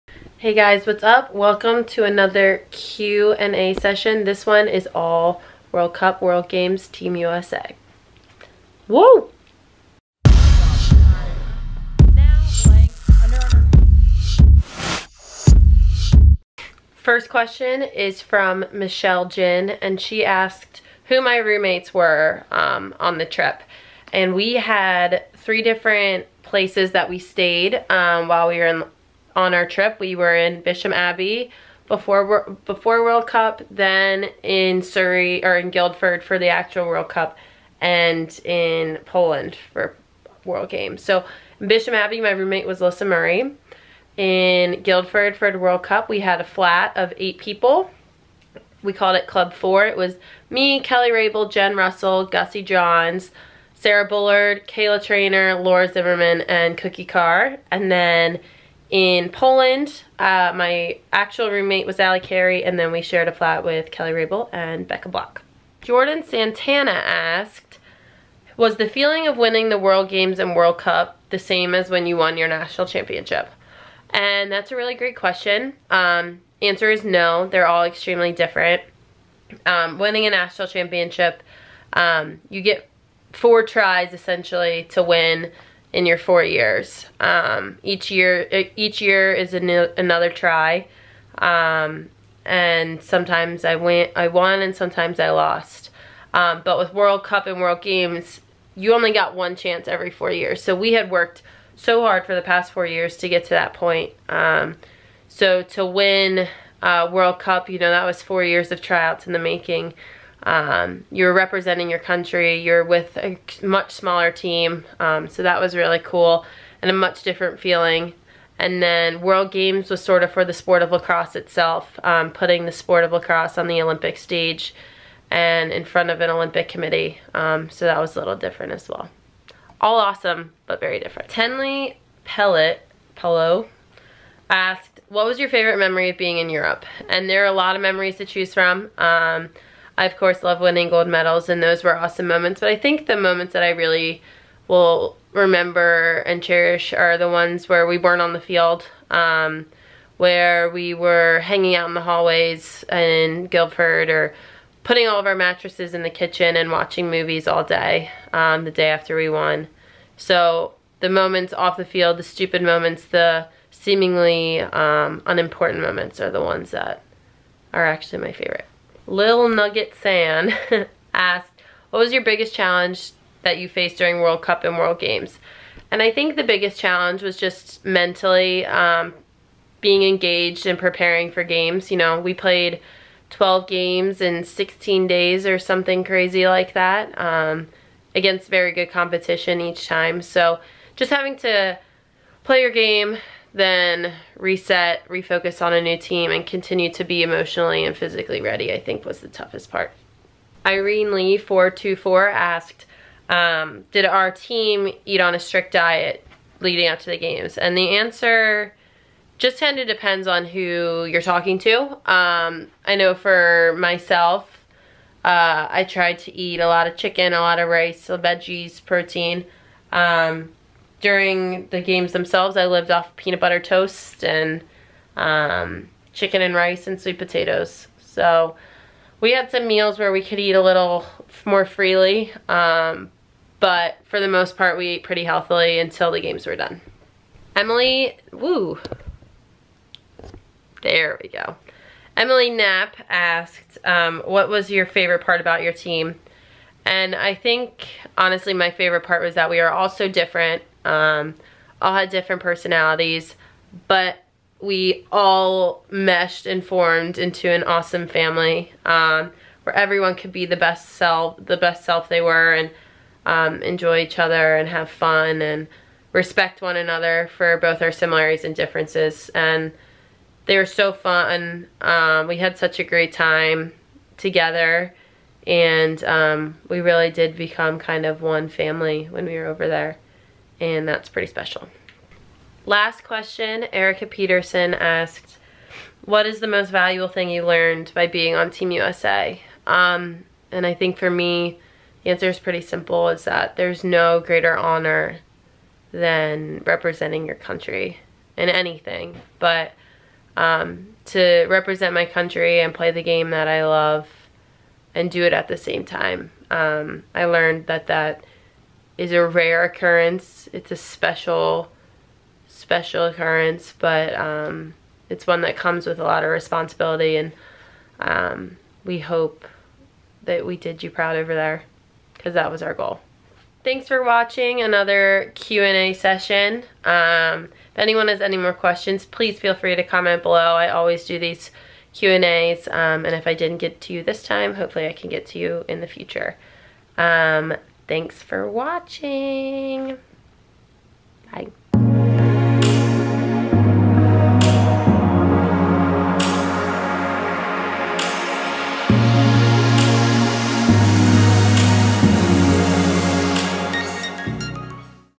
National Championship vs World Cup? | Q&A - Ep. 6
Answering your questions about World Cup and the World Games.

Follow me on the social medias...